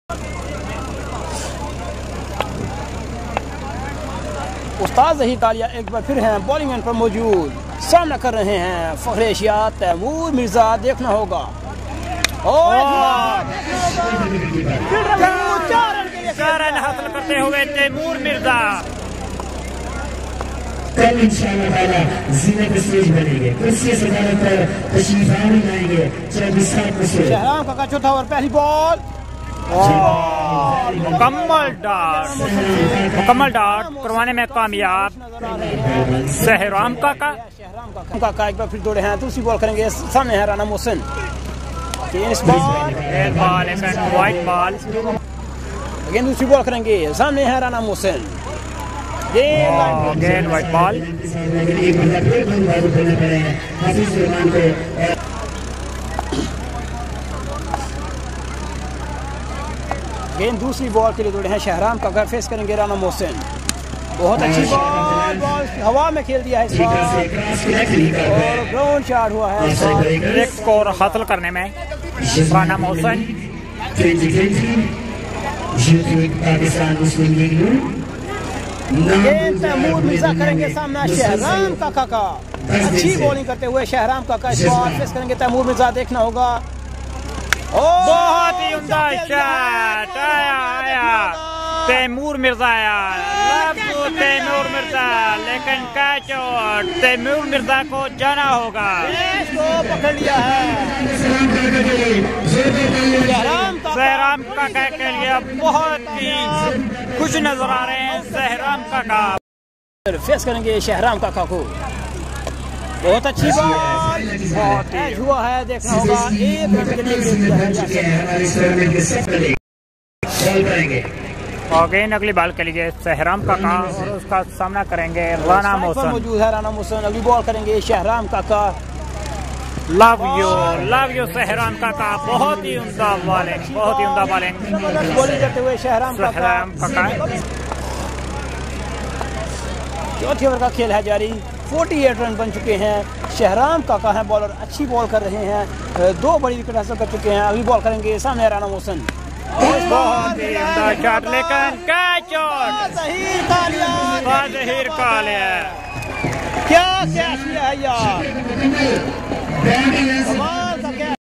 YPL 2021 | Match 1 | Waseem Lefti,Sanam Iqbal,Rana Muhsin Vs Tamoor Mirza bitting | sky sports 41
Yazman Premier League 2021

YPL 2021 | Match 1 | Waseem Lefti,Sanam Iqbal,Rana Muhsin Vs Bahadar Lefti,Aziz Kandhro,Ahmad Lefti